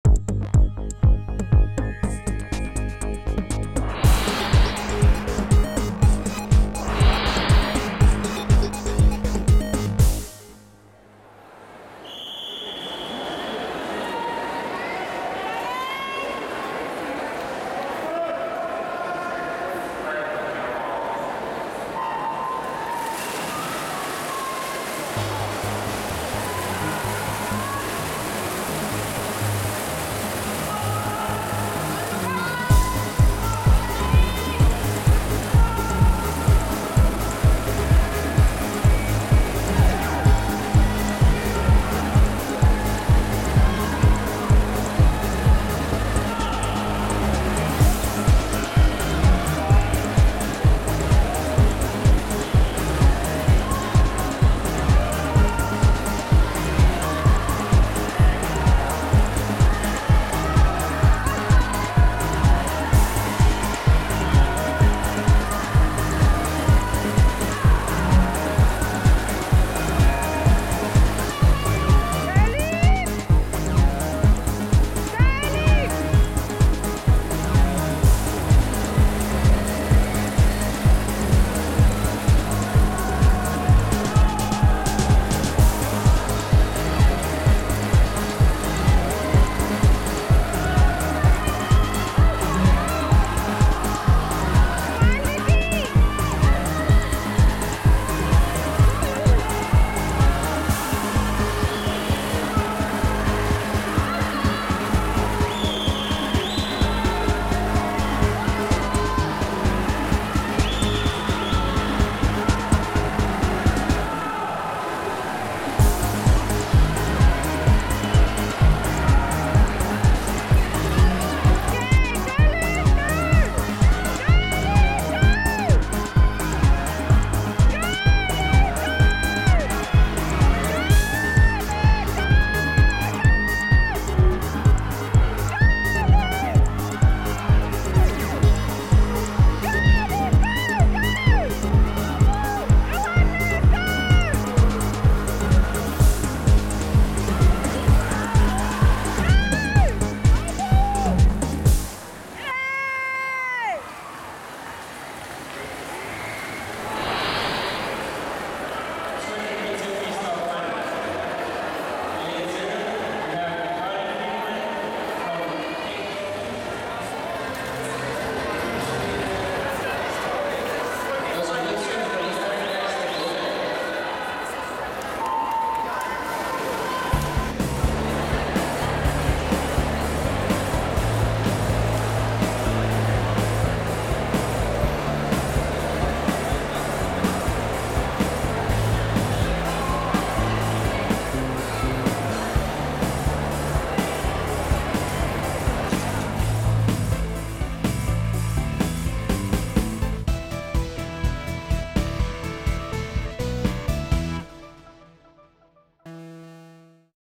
Luke Erwee
Luke Erwee - 1st Place, 200m Free,Level 3 - S.A Champs 2013, Durban, South Africa